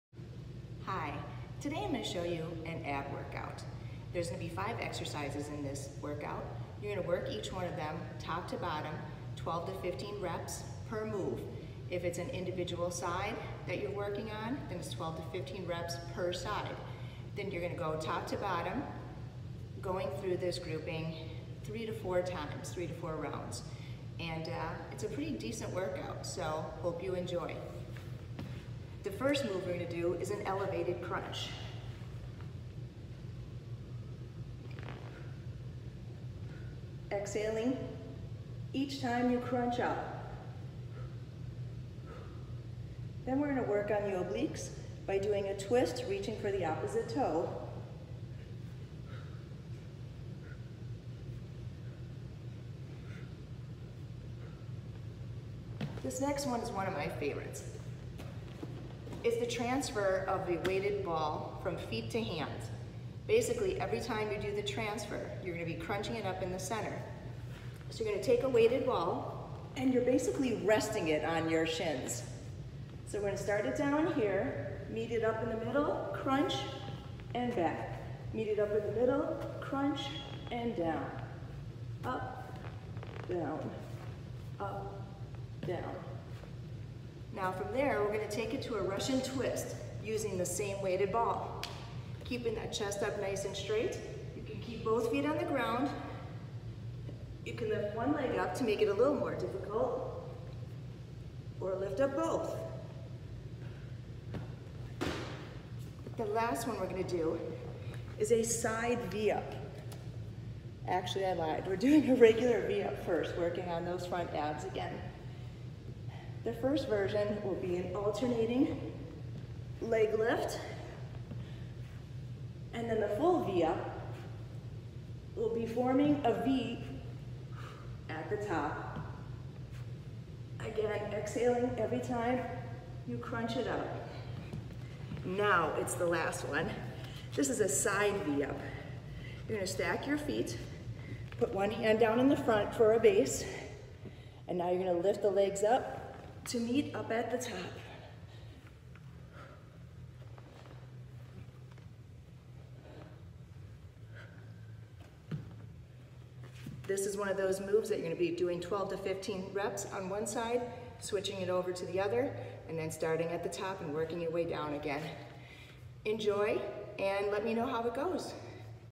AB Workout With Ann from Pleasant Valley Tennis & Fitness Club
Pleasant Valley Tennis & Fitness Club instructor Ann Treinen shows us a great Ab workout that you can do at home or anywhere.